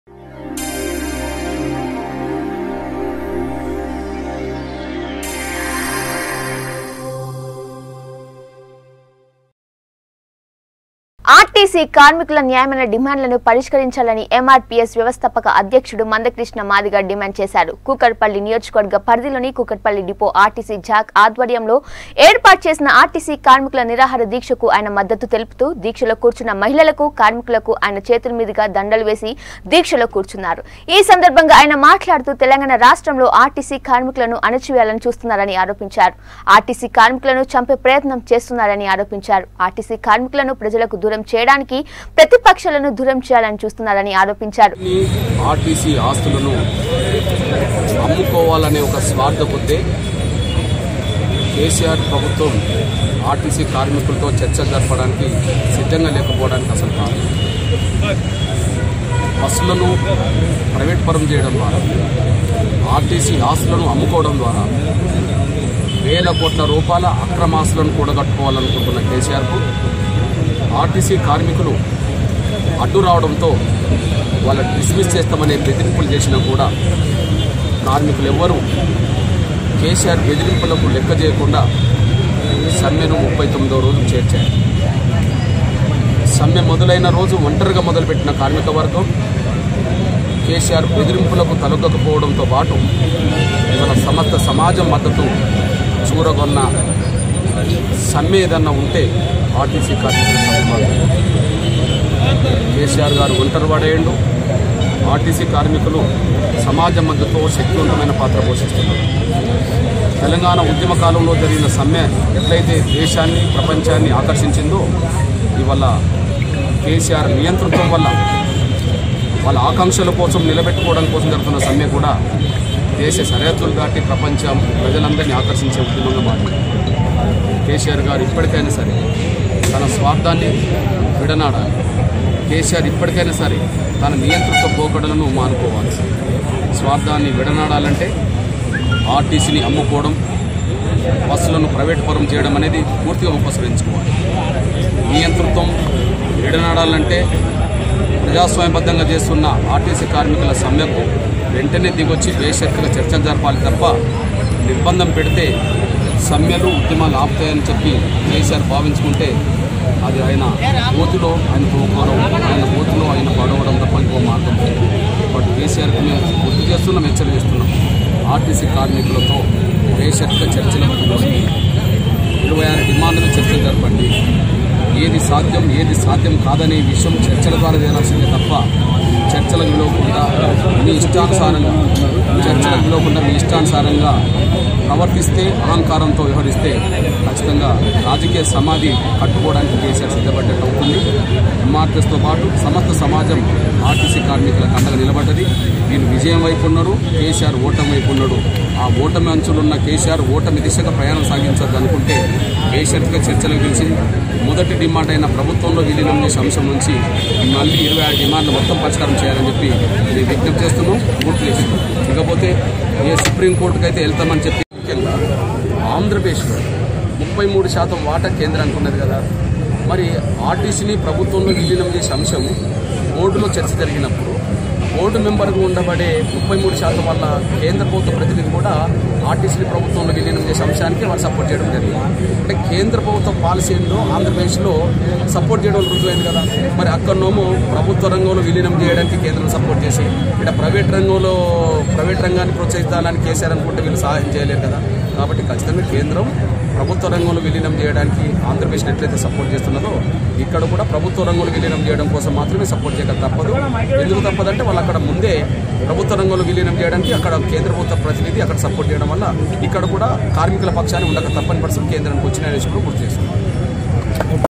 MRPS Leader Manda Krishna Comments on CM KCR over RTC Strike | Kukatpally | Star9 News
MRPS Leader Manda Krishna Comments on CM KCR over RTC Strike | Kukatpally | Star9 News  || Voice Of Truth